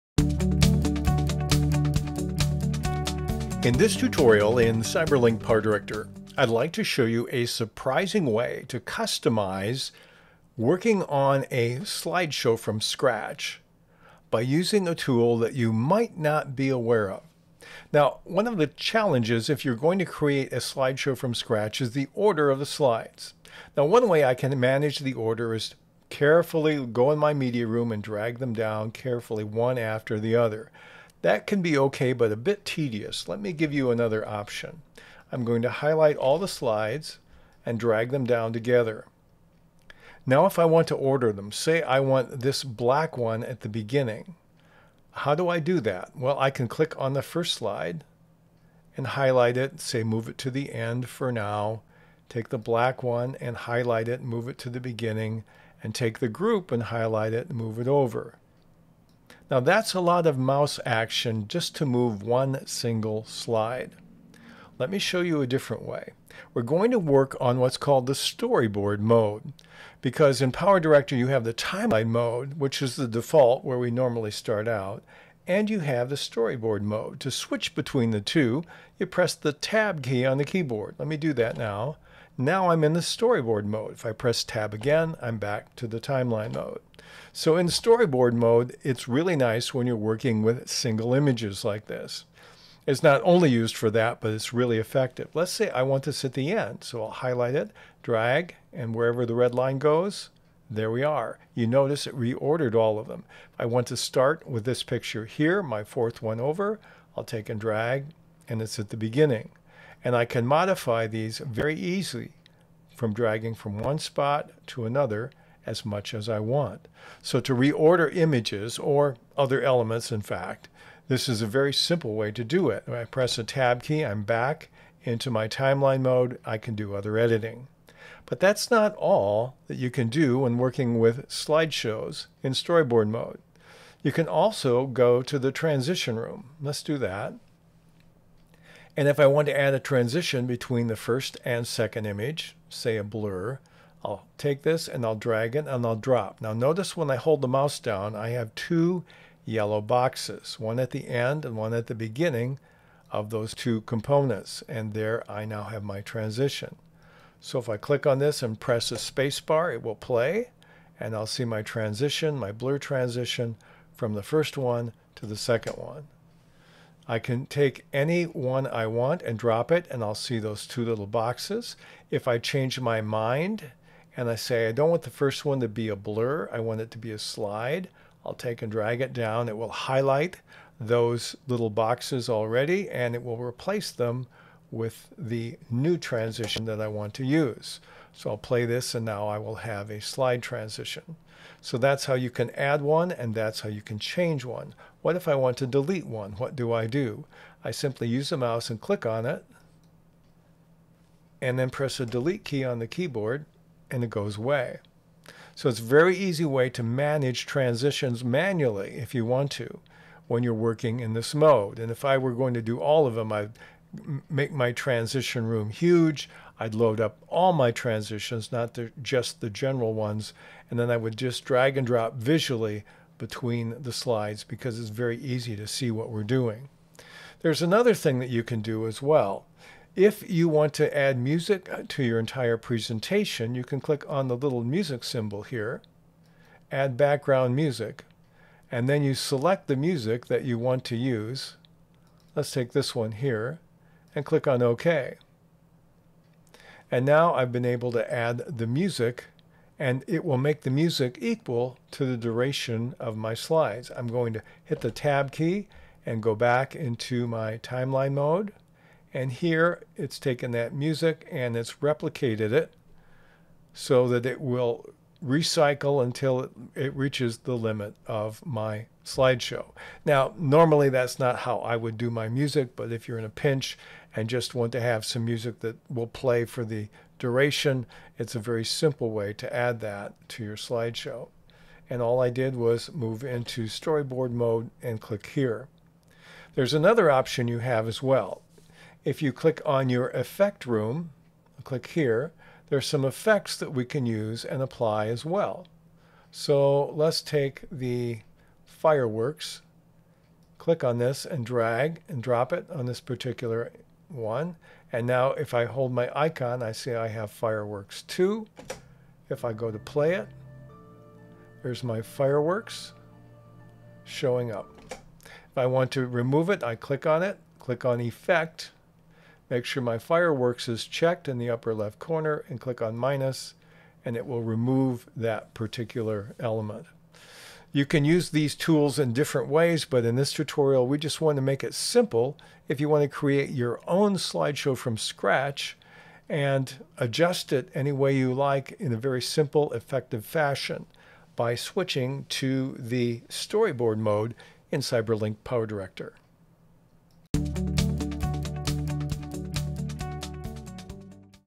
PowerDirector - A surprising way to customize a slideshow
If you are designing your own slideshow, it can be difficult to reorder slides, change transitions, and accomplish other tasks. We show you an easy tool that makes applying these modifications easier than you might think.